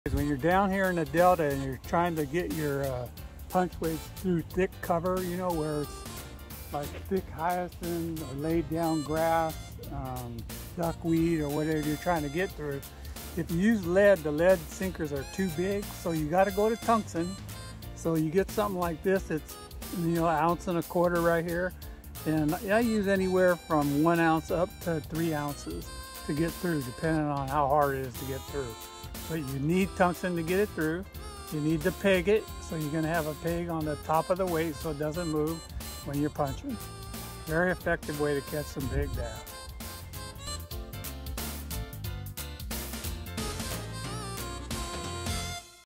Why Use Tungsten Weights? @AlanFong​
Tungsten weights are a must when pitching and flipping in heavy cover such as duckweed, hyacinth and wood! @AlanFong